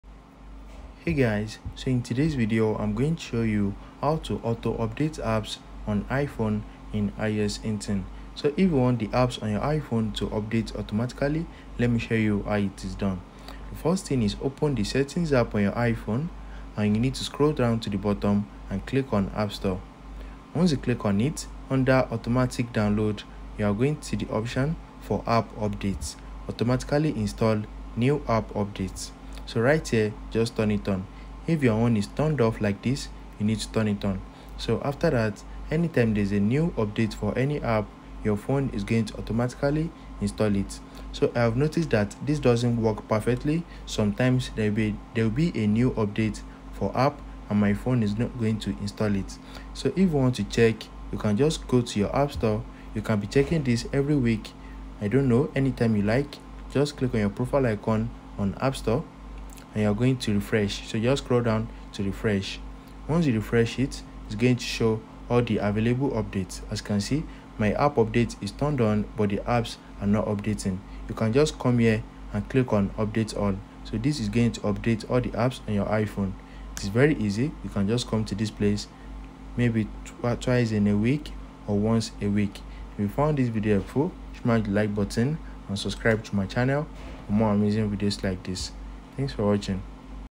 How to Auto Update Apps on iPhone - iOS 18
Learn how to auto update apps on your iPhone with the latest iOS 18 update! This tutorial will guide you through the steps to enable automatic app updates, ensuring your apps are always up-to-date with the latest features and security improvements. Perfect for iPhone users who want to save time and keep their apps running smoothly. Follow our easy instructions to set up auto updates on your iPhone today.